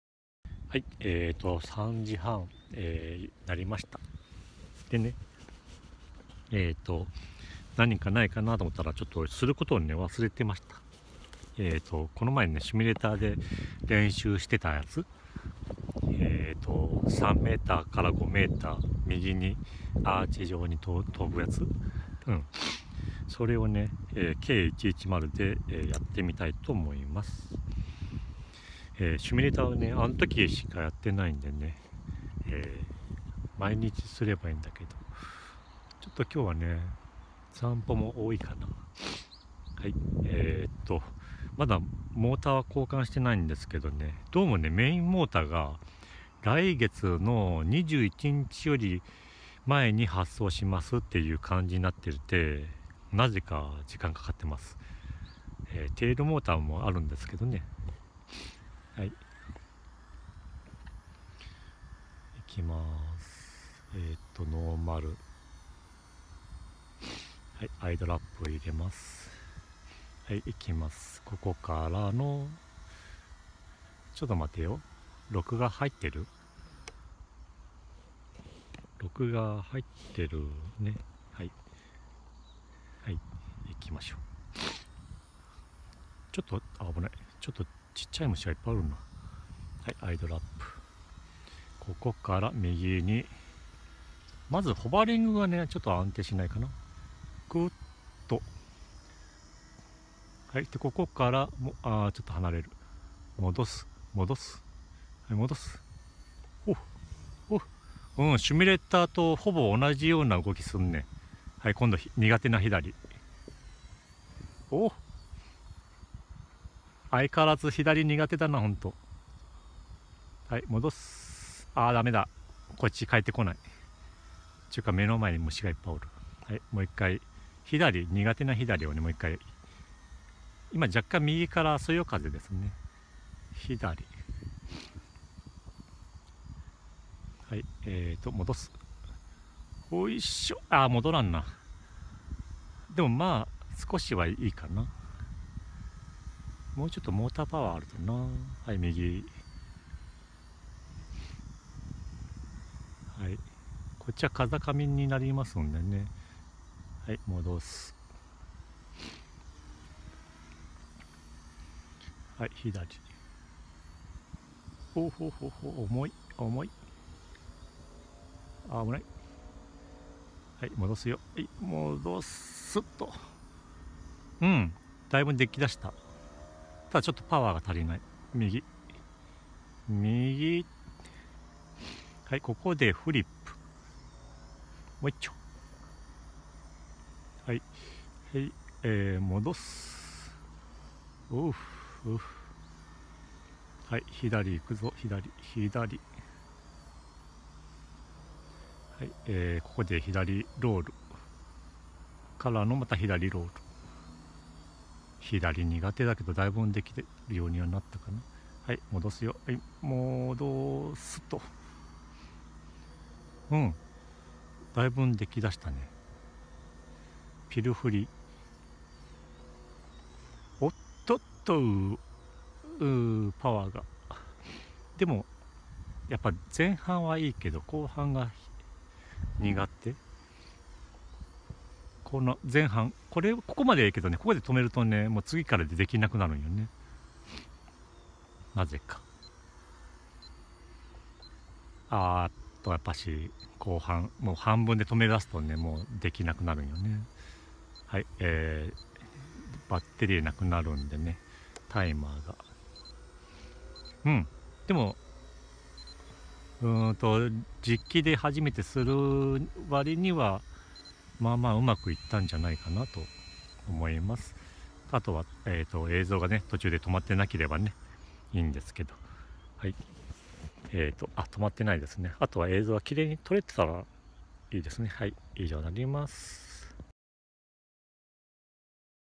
ラジコンヘリ K110 遠めのサイドフリップ
シミュレーターで練習した遠めのサイドフリップを実機でしてみました
やはりというかシミュレーターと同じように止めが揺れるε-(´∀｀; )